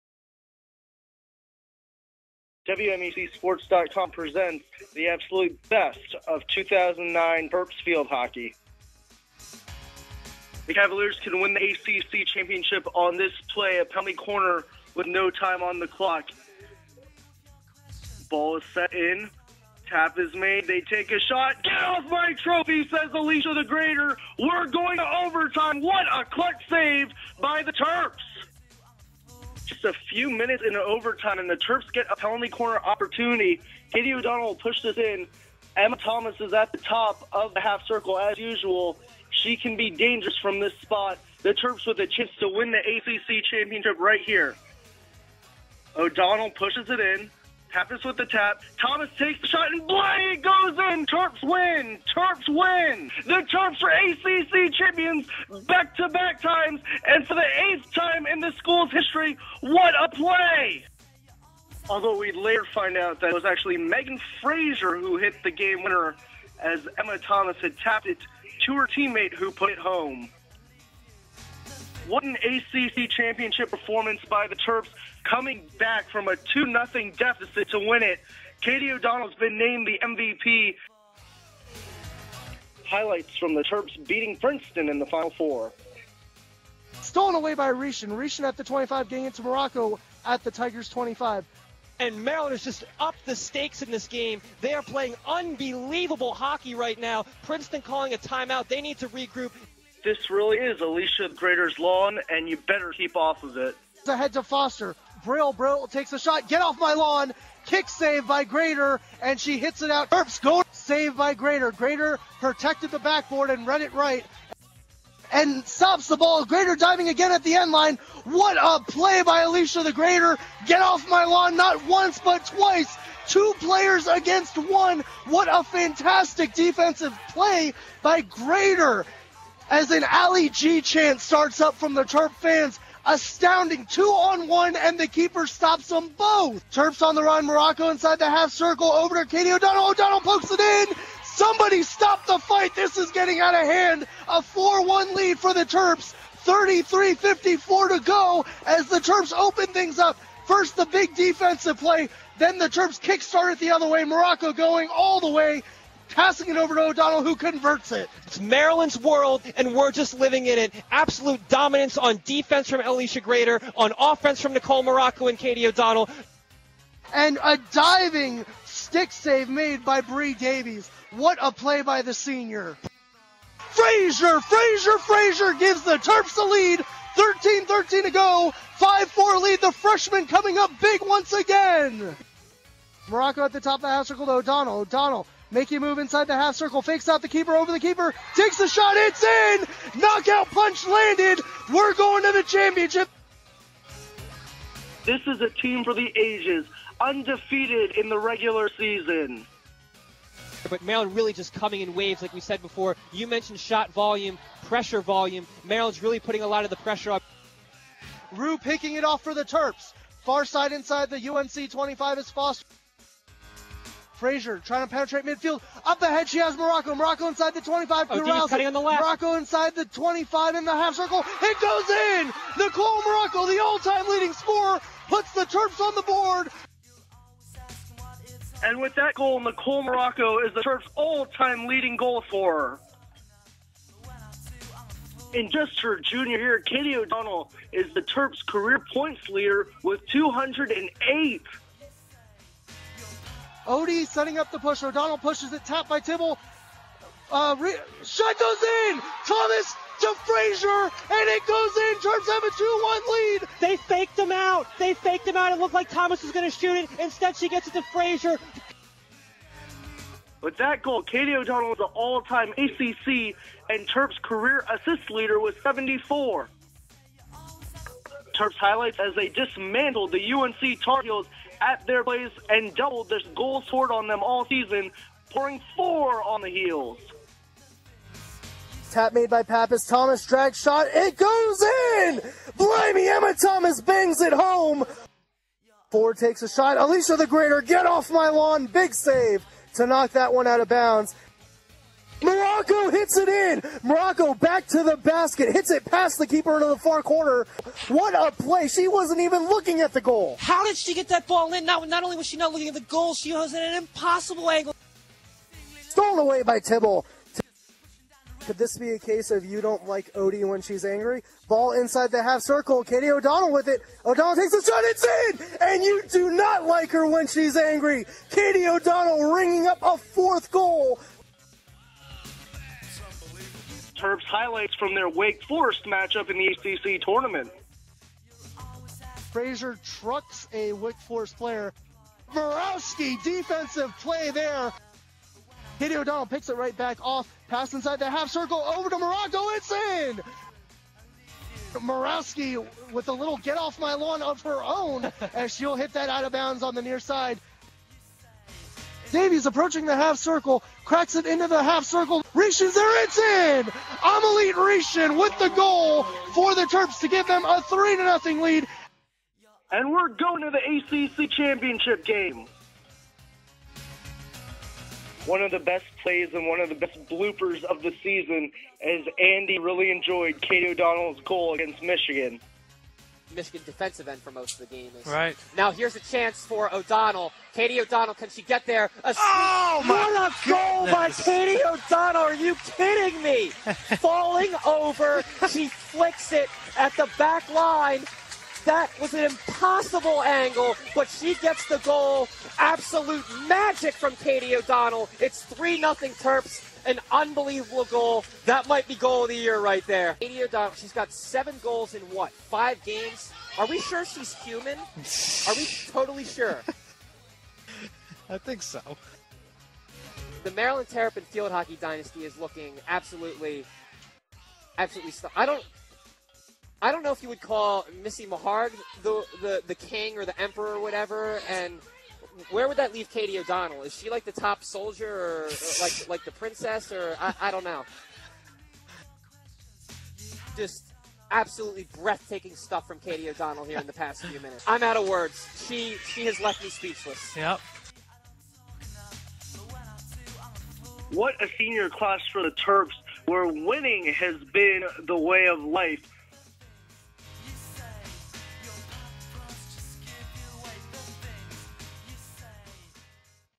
Terps Field Hockey 2009 highlights
Maryland Terrapins field hockey highlights from the 2009 season, Brian Kapur and Andy Gripshover are on the call. All photos also courtesy of Brian Kapur.